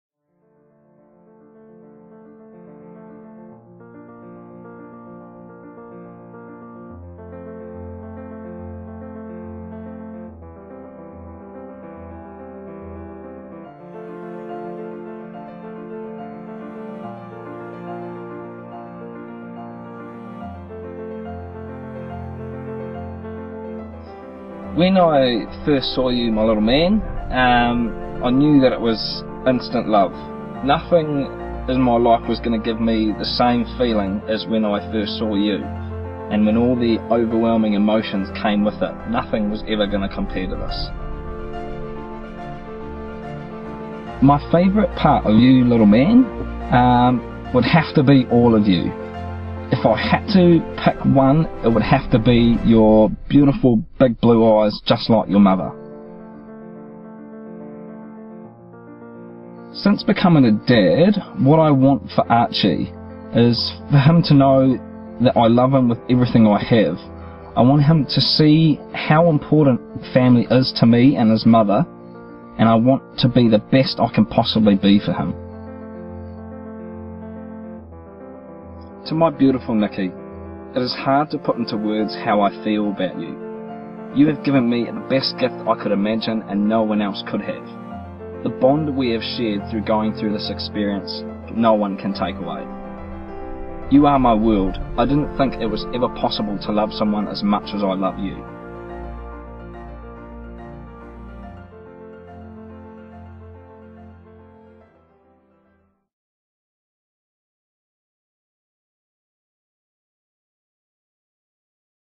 BEHIND THE SCENES Newborn - Moemoea Collective - Photographer Tessa Russell
Photoshoot with Photographer Tessa Russell of Moemoea Collective.
Portrait Photographer. Wellington & Palmerston North, New Zealand.
Thanks to Nicky & Andy for letting me share your session and my sister Hollie for your video skills.